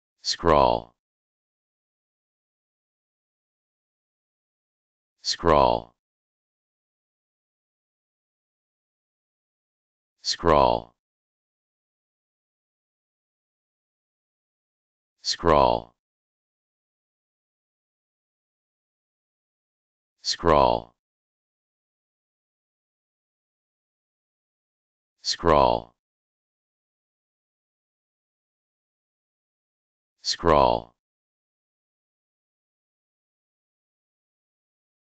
How to Pronounce Scrawl
Scrawl - your guide to pronouncing this word, brought to you for free by uPronounce.